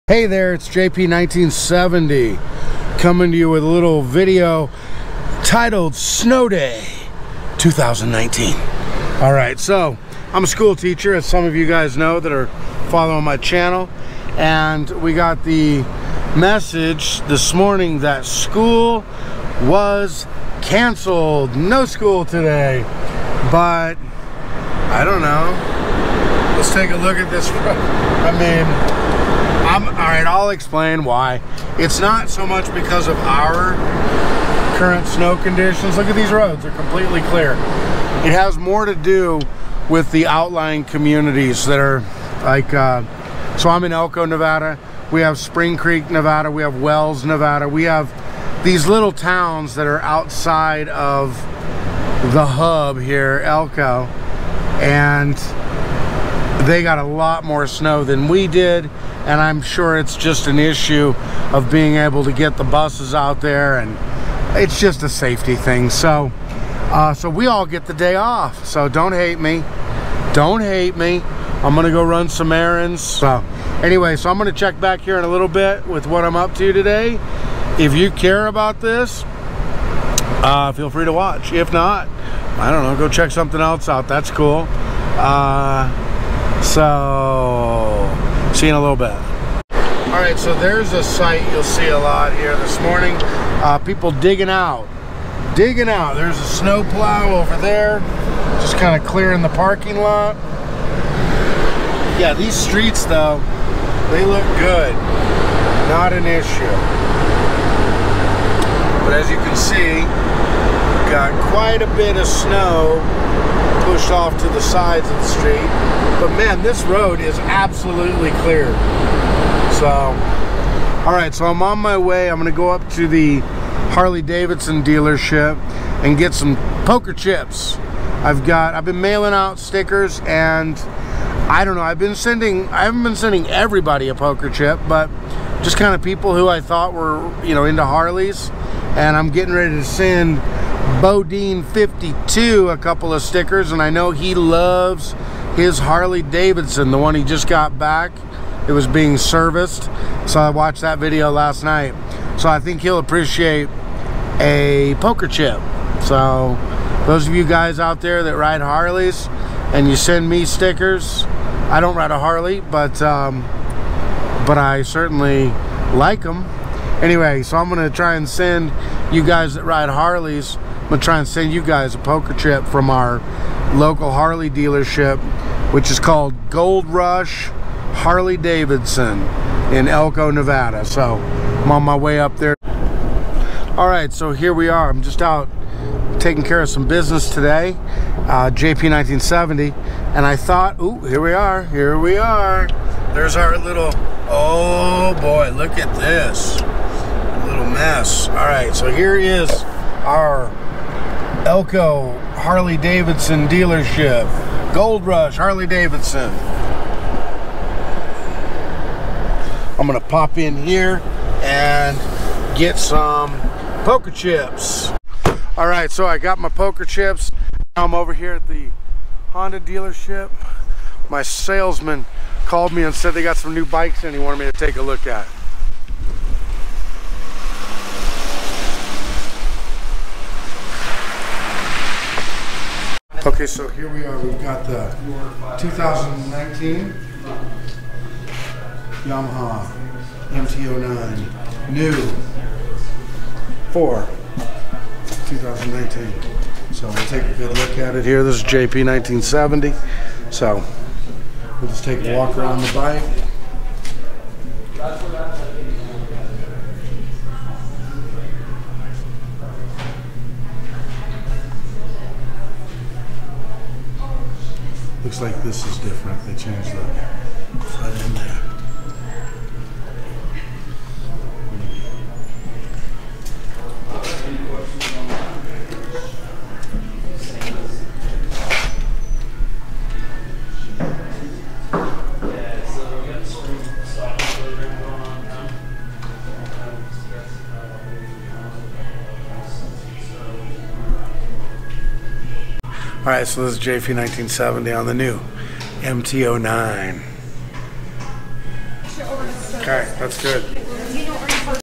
Snow Day || Gold Rush H-D || 2019 Yamaha MT-09
No school today, so I decided to go buy some H-D poker chips and take a look at the new Yamaha MT-09.